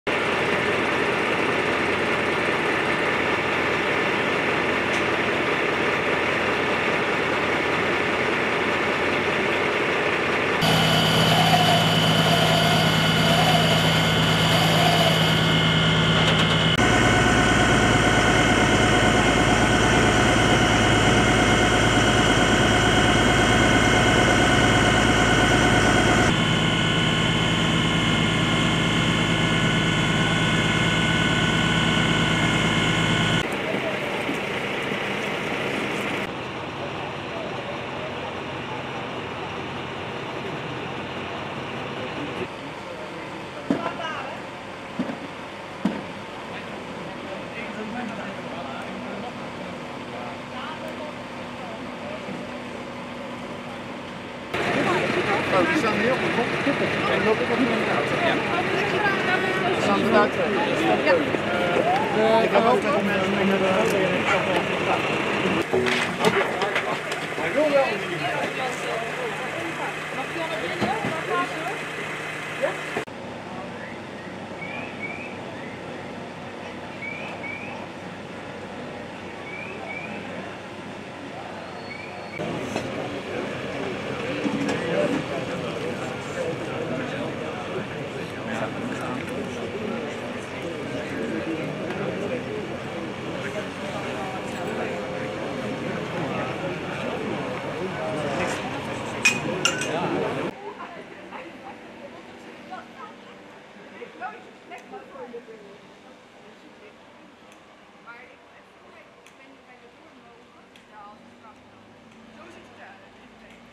Grote Brand Salamanderveen, dieren gered twee personen naar het ziekenhuis
In een woning aan de Salamanderveen heeft een flinke brand gewoed. De hulpdiensten schaalde op naar grote brand om meer voertuigen ter plaatse te krijgen. De brandweer heeft geprobeerd om overslag naar andere woningen te voorkomen.

Omstanders geven aan dat de vlammen metershoog boven de woning uit kwamen. De brandweer heeft om goed zicht te krijgen op de brand een drone ingezet. Met de drone kan de brandweer de hotspots zien en aangeven waar extra aandacht nodig is voor het blussen. De aangrenzende woningen zijn enige tijd ontruimd geweest. Dit omdat niet duidelijk was of de brand was overgeslagen naar naastgelegen woningen.

Er zijn vier mensen nagekeken door de ambulance. Hiervan zijn twee mensen vervoerd naar het ziekenhuis. Dit omdat deze personen veel rook hadden ingeademd. De dierenambulance heeft 5 slangen, 2 honden, 2 katten en 1 hagedis opgevangen.

Omdat er rook van het dak bleef afkomen heeft de brandweer delen van het dak gesloopt. Op deze manier kon er gerichter worden geblust. Inmiddels is het sein brandmeester gegeven.